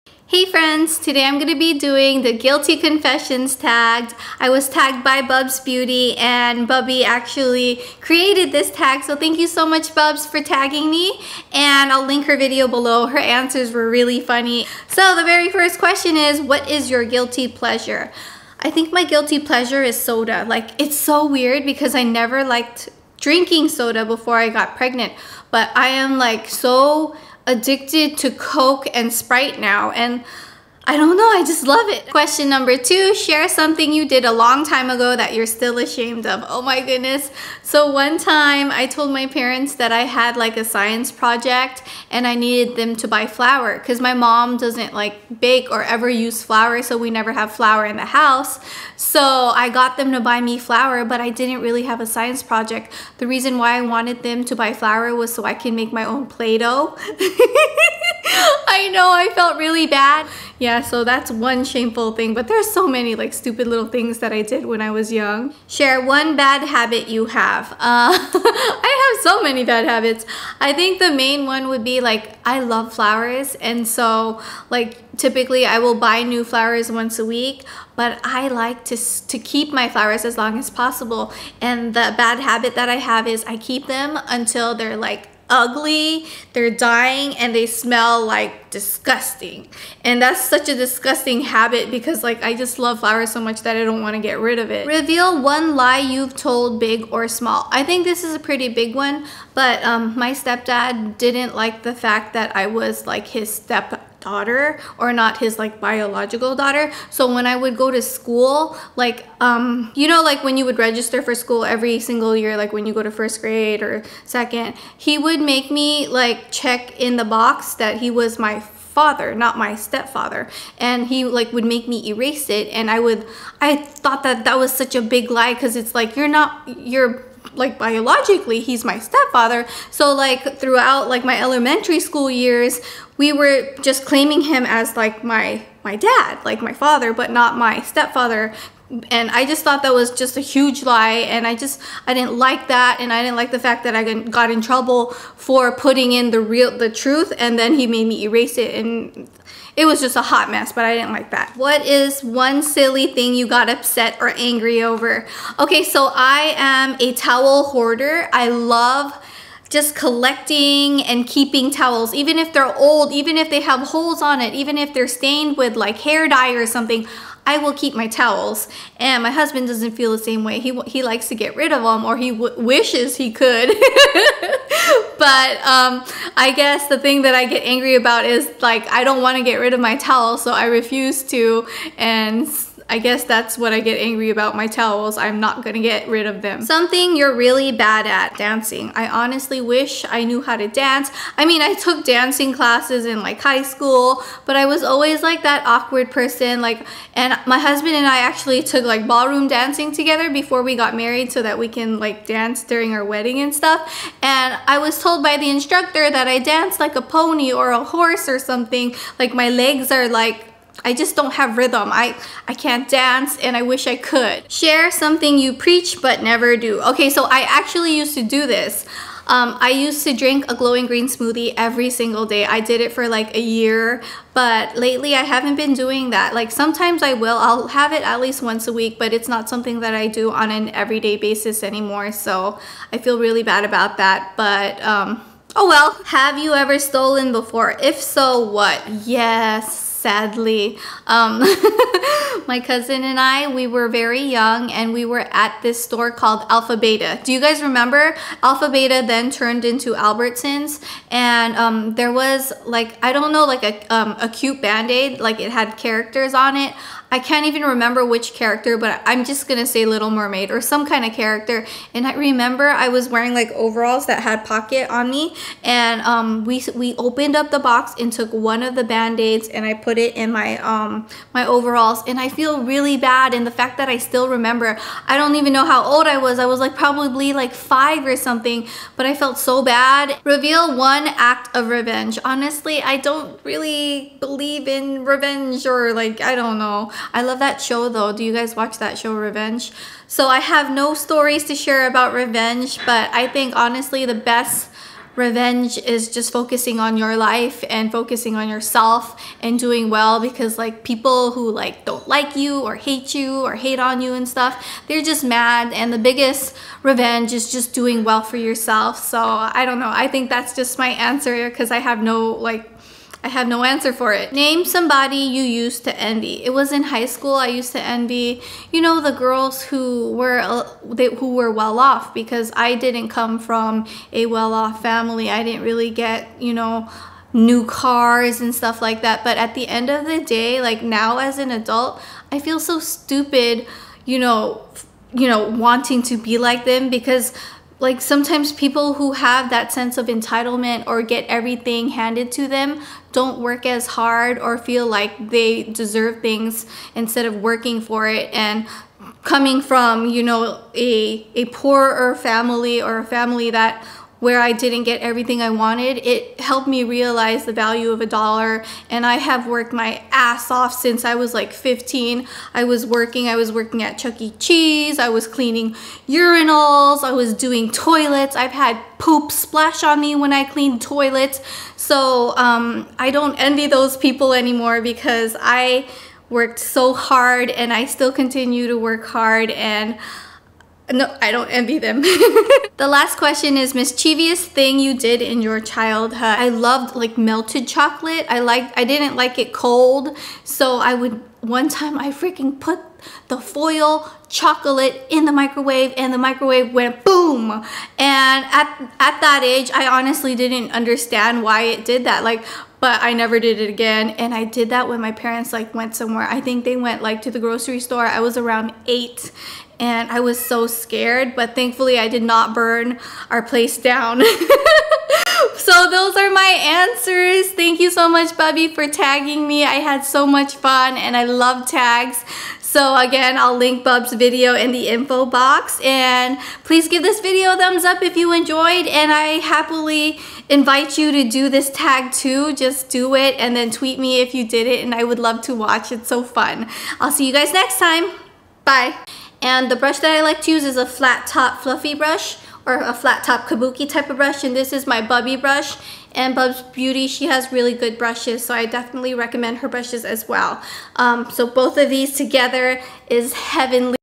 Guilty Confessions!
✿ Thank you Bubbi (Bubzbeauty) for tagging me!
I tag EVERYONE to participate! :D

✿ Question of the day: What was the biggest lie you ever told?

✿ GUILTY CONFESSIONS TAG
What is your guilty pleasure?
Share something you did a long time ago that you're still ashamed of.
Share one bad habit you have.
Reveal one lie you've told. Big or small.
What is one silliest thing you got upset or angry over?
Something you're really bad at:
Share something you preach but never do.
Have you ever stolen before? If so,what?
Reveal one act of revenge.
Name somebody you used to envy.
Mischievous thing you did in your childhood

APRIL'S INSTAGRAM: @aprilathena7
JUSTIN'S INSTAGRAM: @advicefromjustin

39 weeks pregnant. For me to look back and remember.